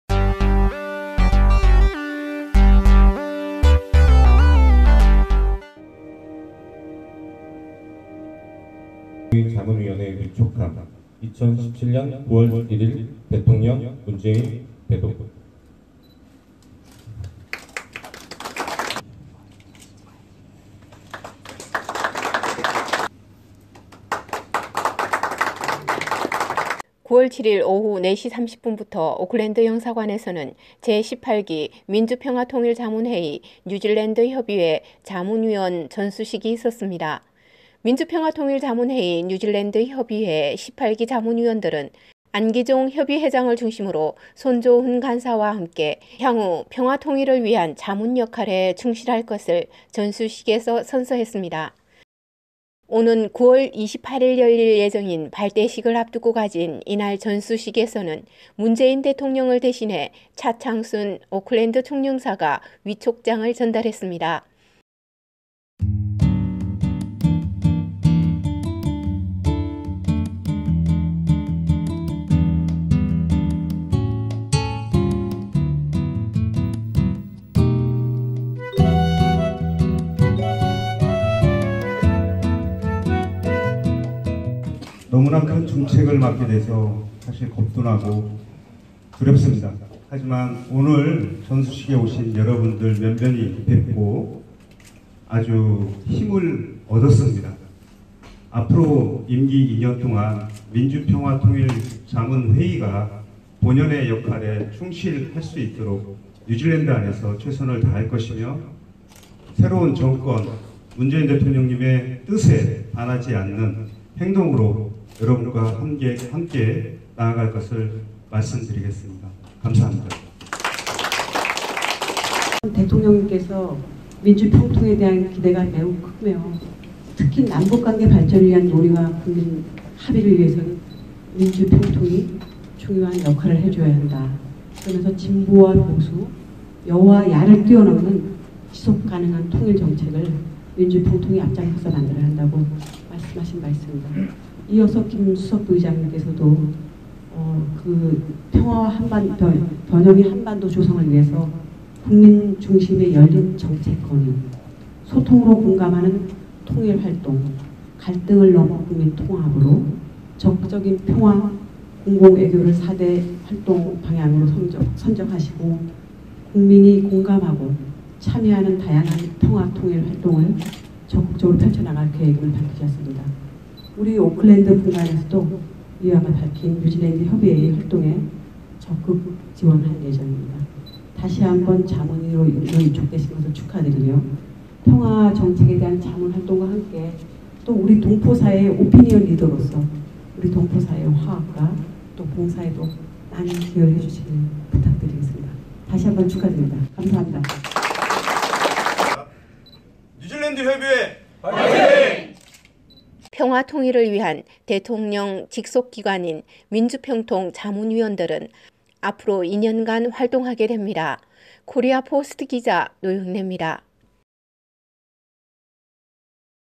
18기 민주평통 뉴질랜드협의회 자문위원 전수식
9월 7일 오후 4시 30분부터 오클랜드 영사관에서는 제 18기 민주평화통일자문회의 뉴질랜드 협의회 자문위원 전수식이 있었습니다.

민주평화통일자문회의 뉴질랜드 협의회 18기 자문위원들은 안기종 협의회장을 중심으로 손조훈 간사와 함께 향후 평화통일을 위한 자문 역할에 충실할 것을 전수식에서 선서했습니다.

오는 9월 28일 열릴 예정인 발대식을 앞두고 가진 이날 전수식에서는 문재인대통령을 대신해 차창순 오클랜드 총영사가 위촉장을 전달했습니다.

평화 통일을 위한 대통령 직속기관인 민주평통 자문위원들은 앞으로 2년간 활동하게 됩니다.